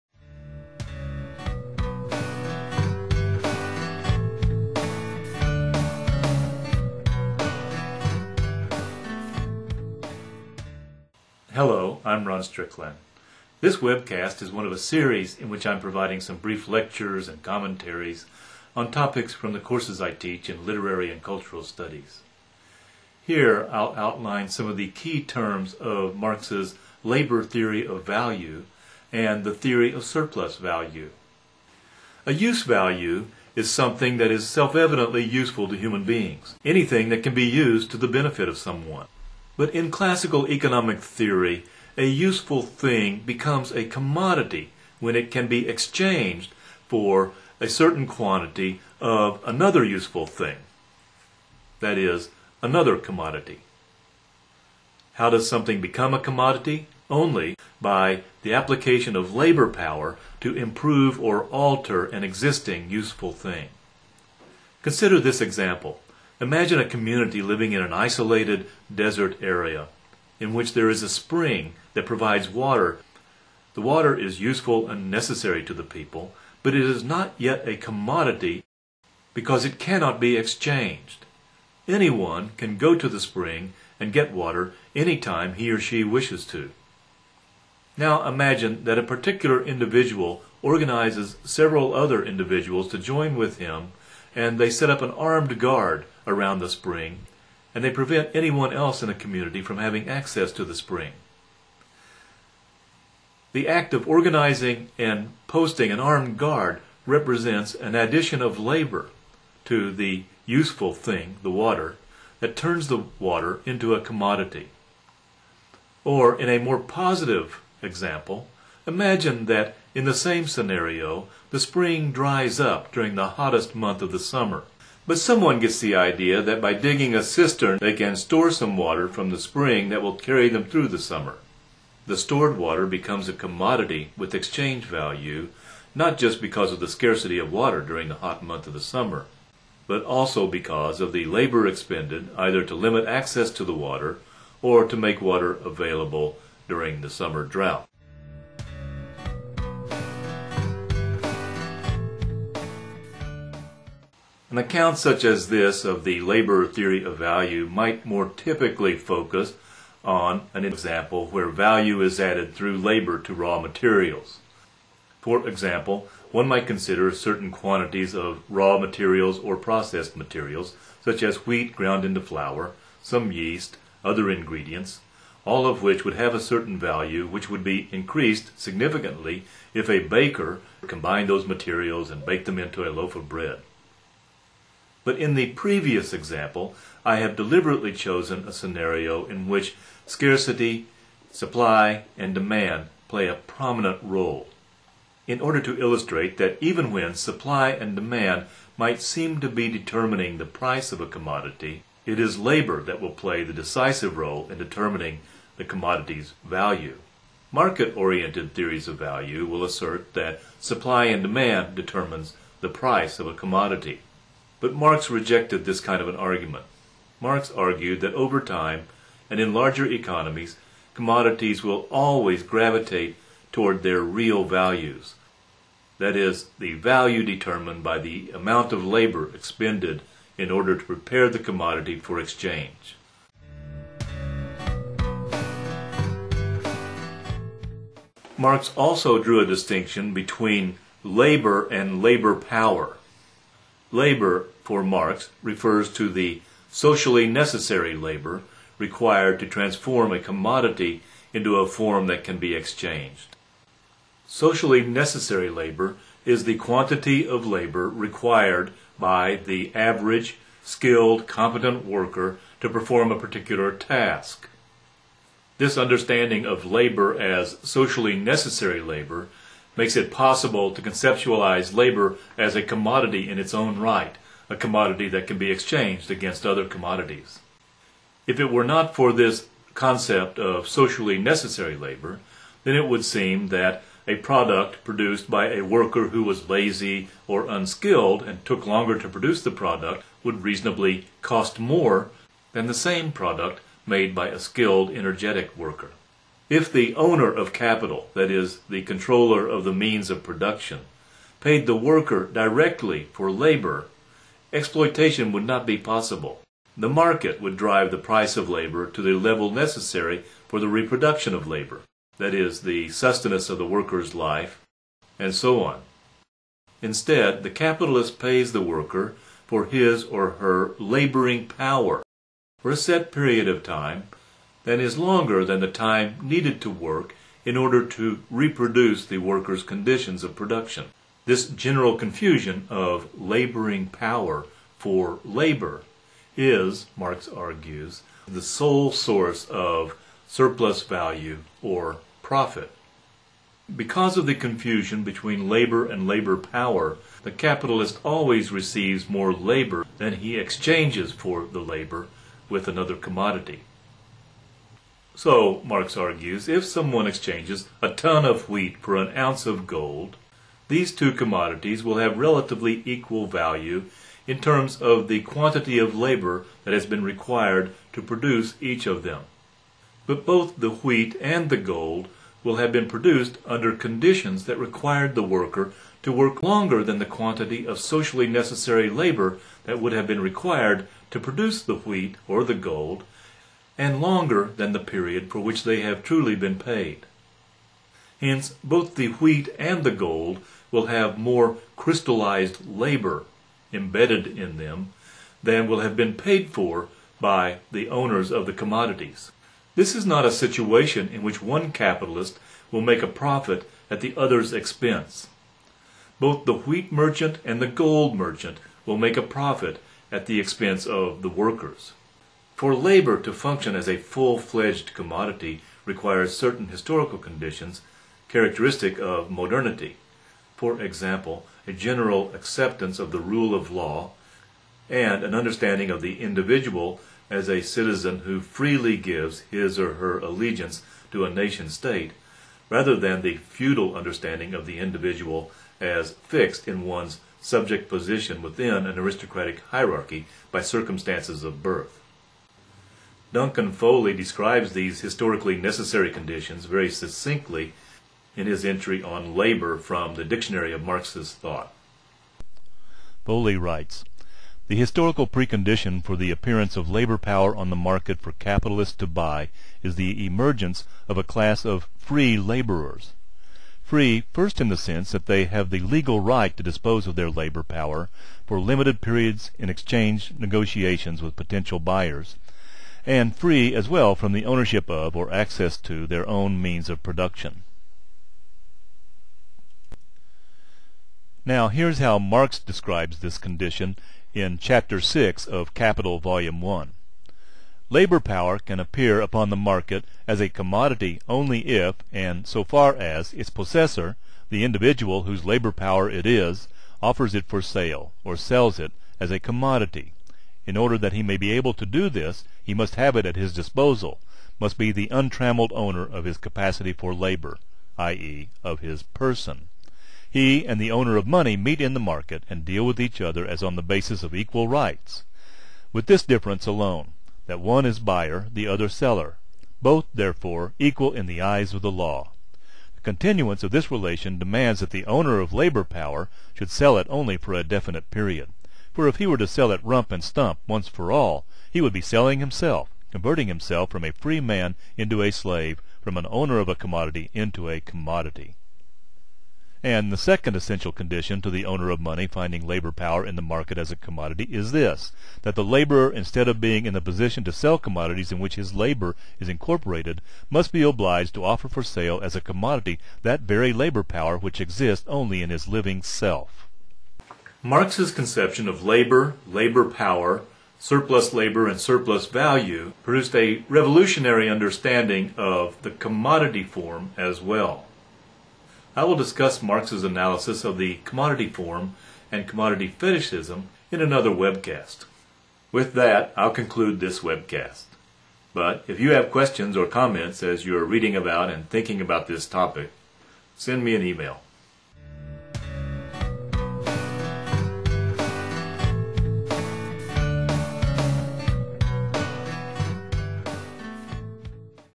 Cultural Theory: The Labor Theory of Value
College course lecture on the labor theory of value.